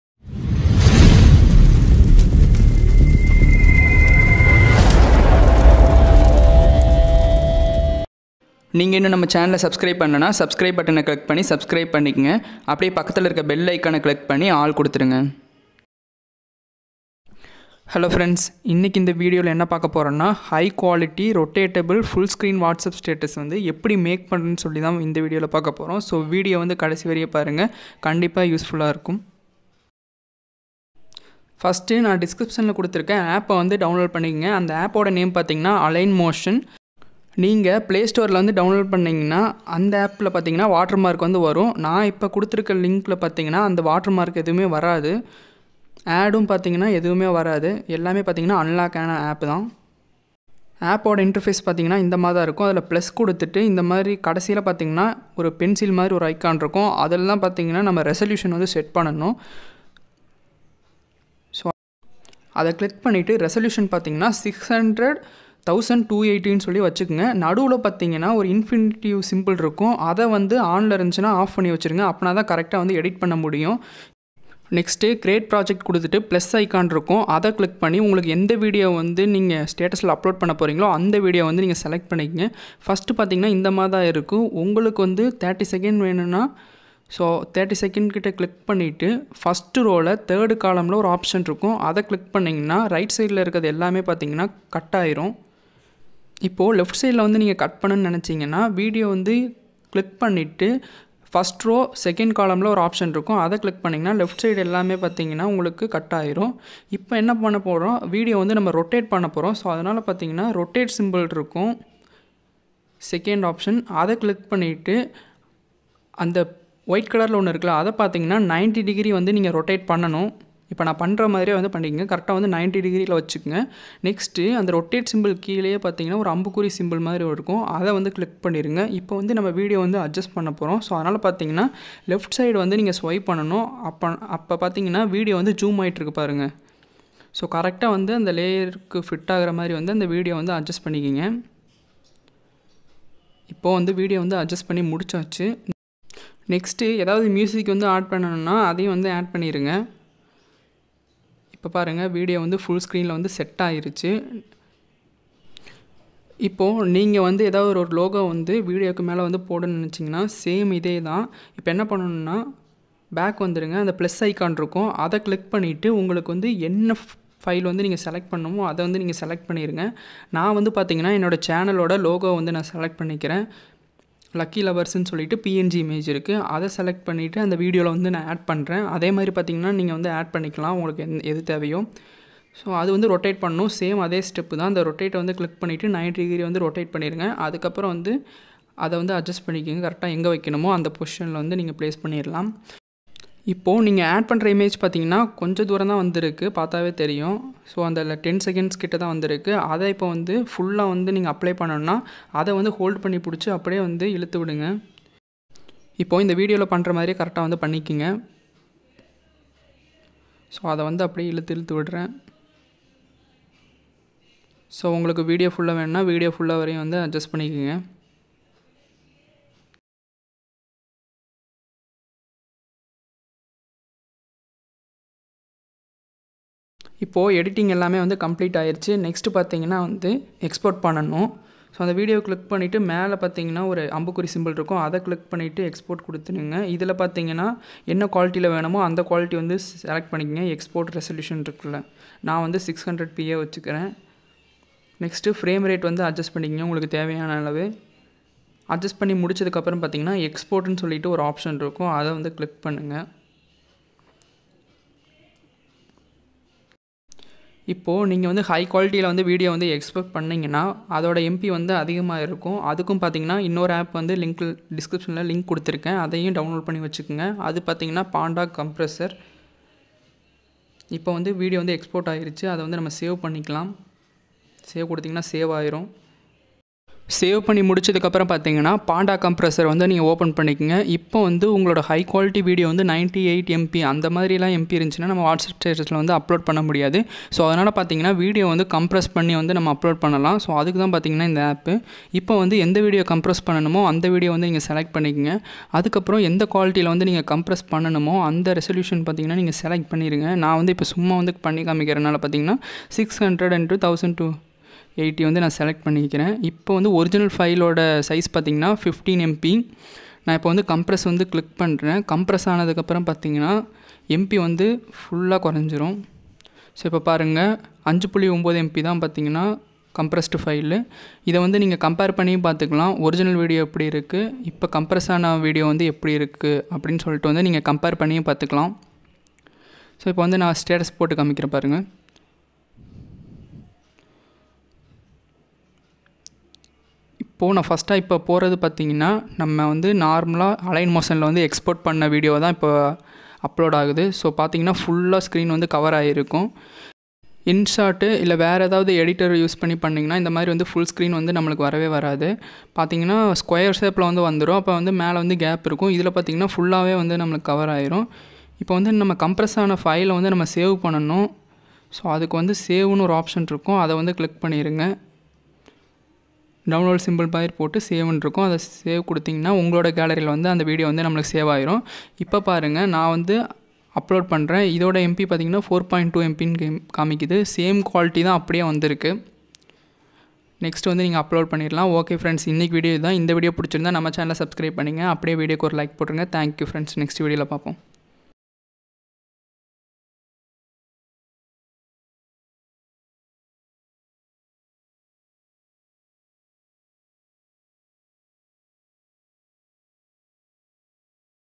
how to make 4k whatsapp status video in tamil | full screen rotated whatsapp status
Tech and electronic videos present